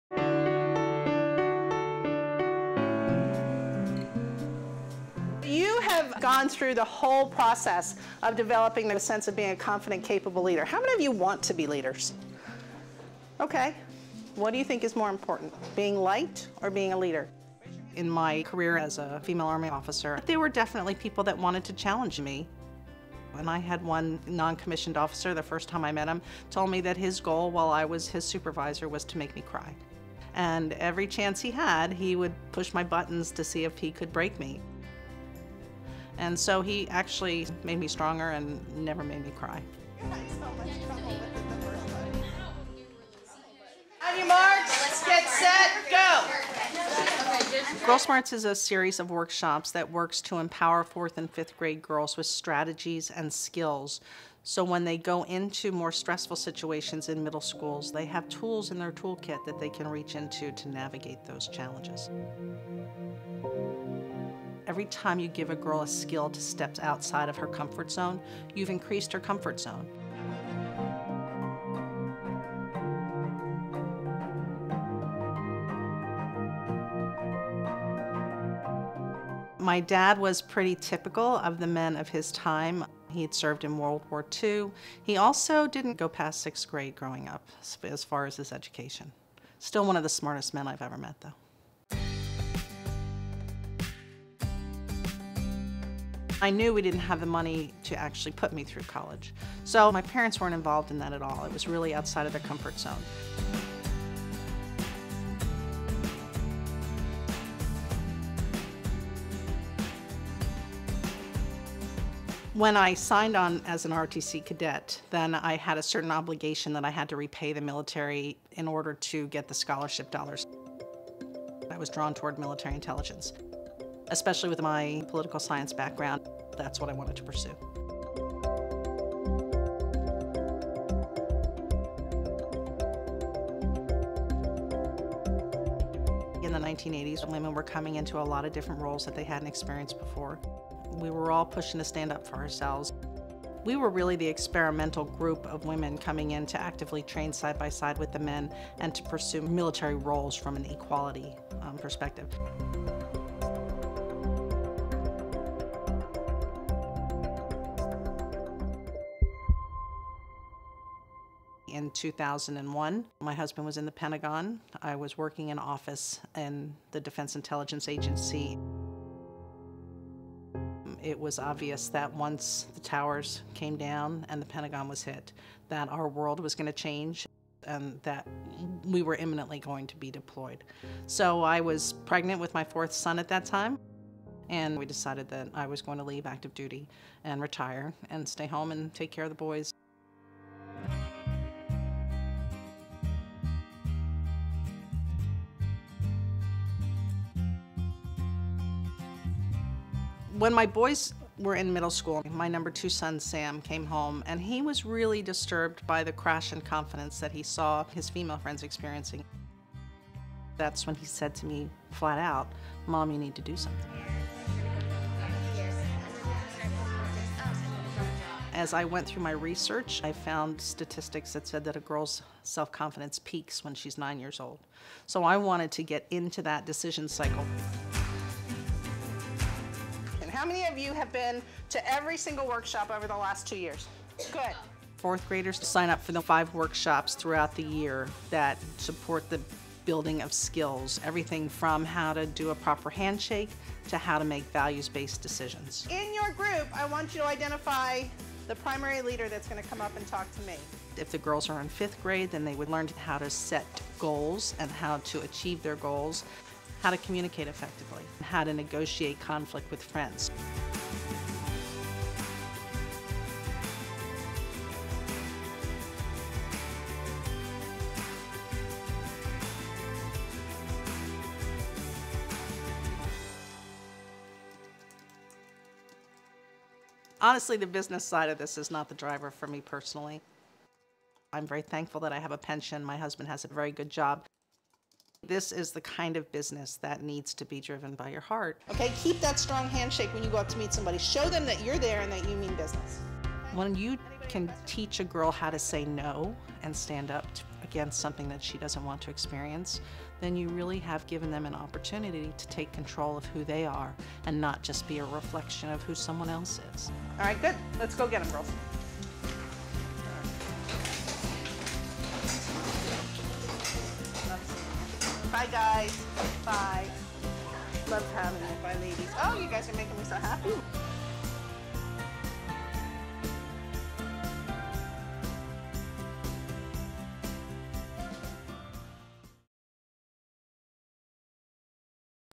Why This Military Vet Is Teaching Confidence to Young Girls | Women Entrepreneurs
Women Entrepreneurs #61 Dianna Flett, a retired U.S. Army lieutenant colonel, has taken on a new kind of public service: teaching leadership and coping skills to 4th- and 5th-grade girls. Her Virginia company, Girl Smarts, offers workshops that help prepare about 800 girls a year for life challenges ahead.

For more videos of women entrepreneurs, news and inspiration on starting your own business, please